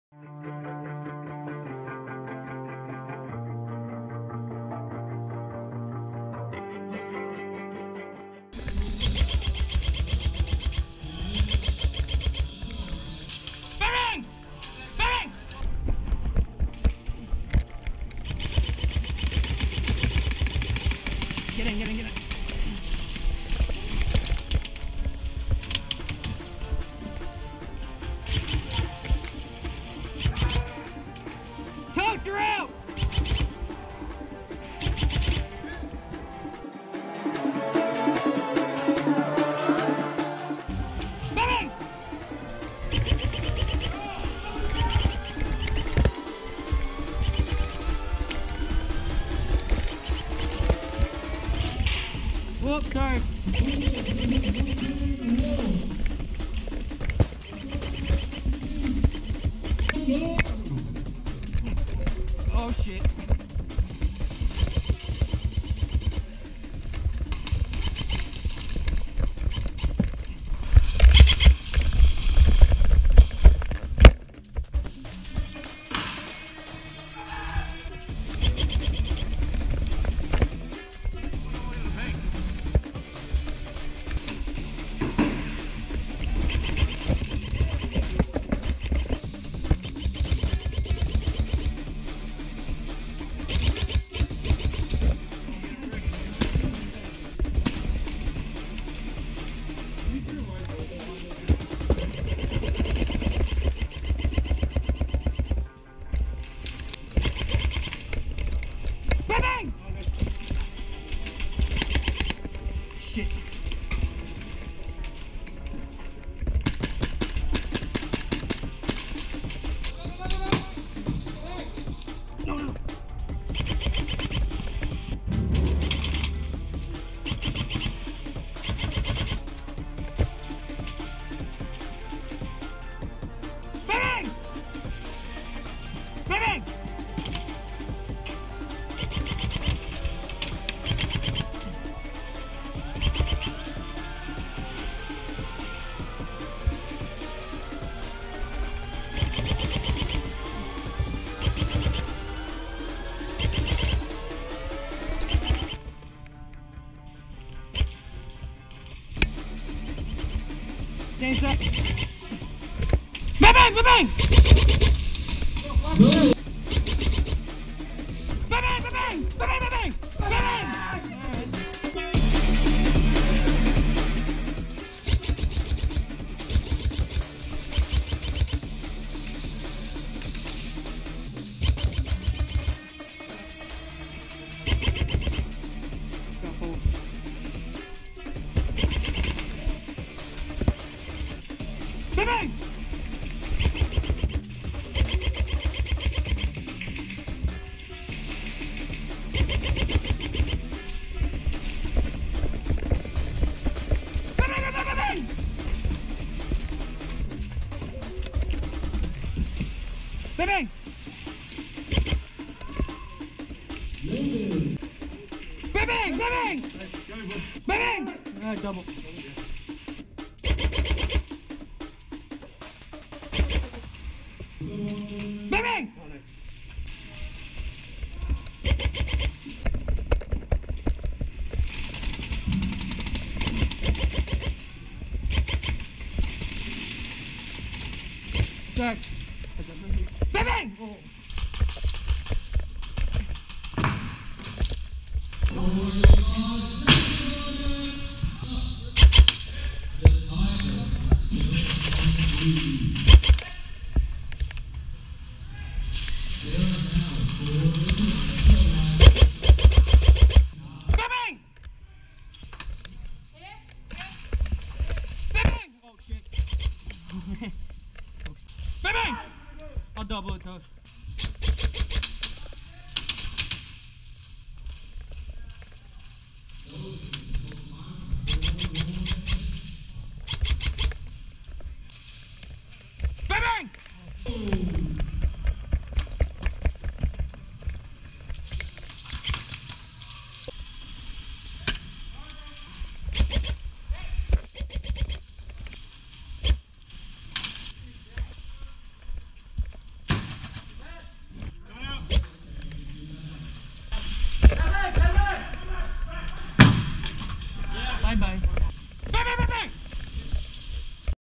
STRYKER AIRSOFT 12/1/18!!!!
Me and the CEA boys going hard at Stryker Airsoft on Saturday running my custom g&g combat machine and kwa usp compact running a few airsoft innovations xl burst grenades !!!